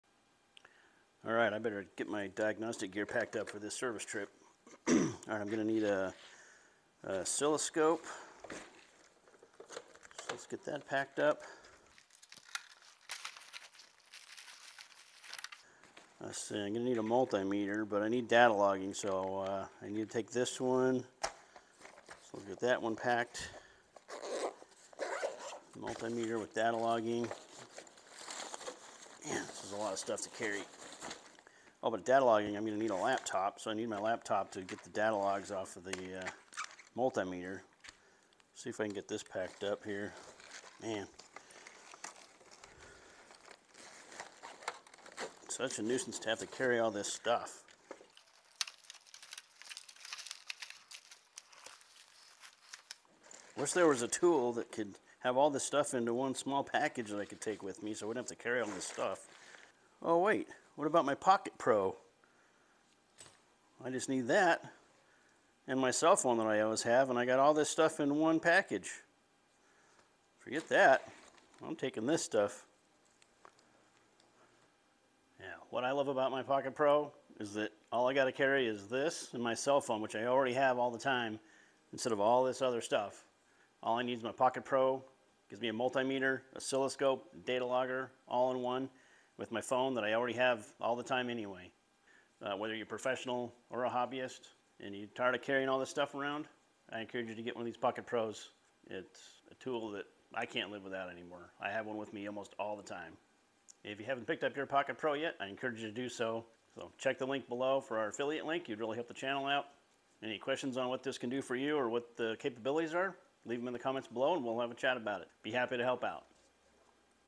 Pokit Pro Combination Multimeter, Datalogger, and Oscilloscope!
Why I love my Pokit Pro!  Do it, do it now!  This thing is awesome!